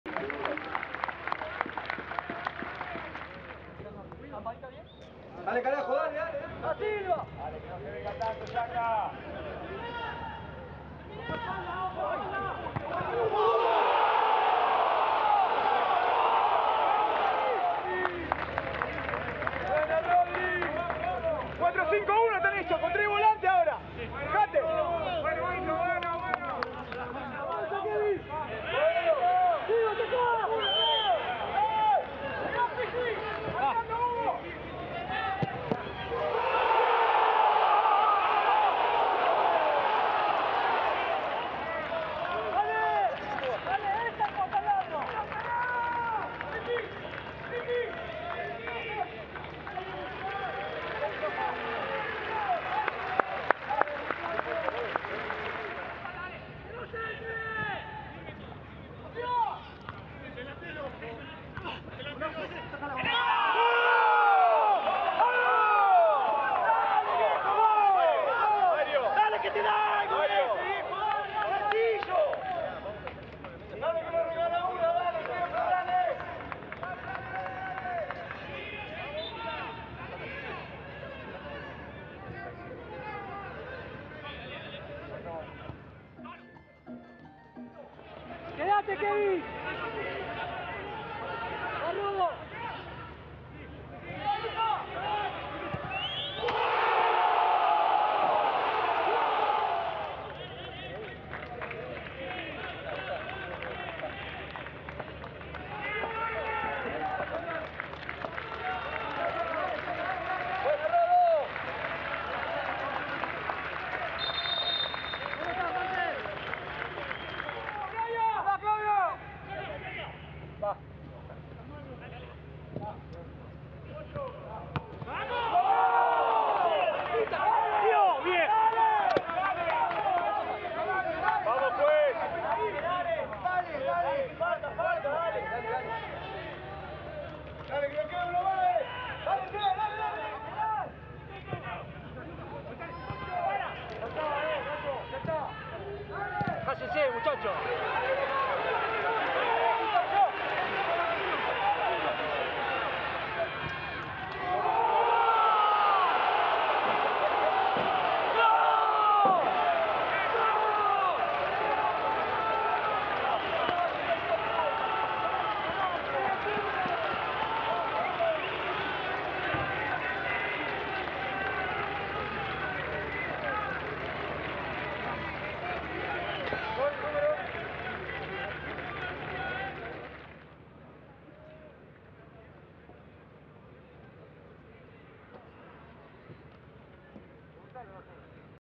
TorneoSocios | Fecha6 | resumen de Defensa y Justicia - Sarmiento
Defensa y Justicia venció 4 a 2 a Sarmiento con goles de Contreras, Pizzini, Rotondi y Barrios por la Fecha6 del TorneoSocios. Torres anotó los dos de la visita.